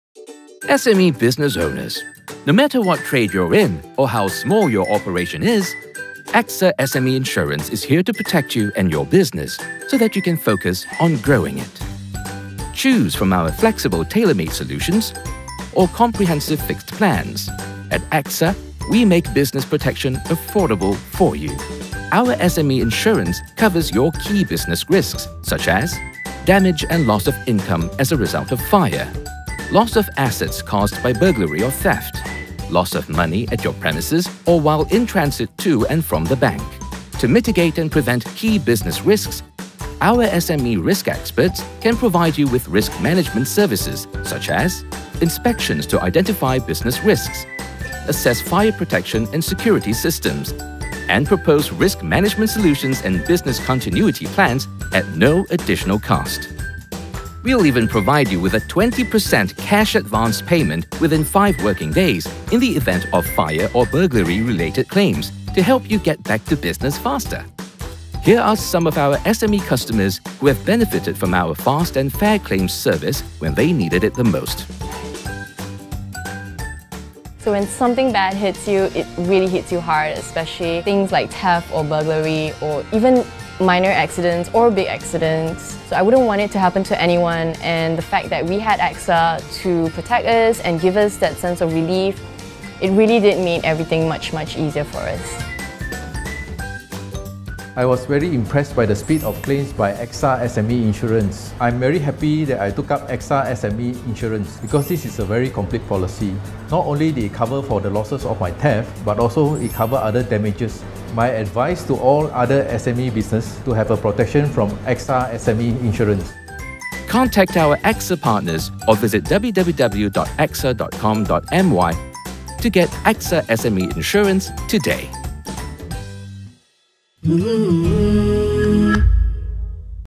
AXA SME Insurance Explainer Video
If you're a small business owner, AXA SME Insurance has got your back!

Check out the fun and colourful explainer video that we have created to introduce the AXA SME Insurance.

Stay awesome!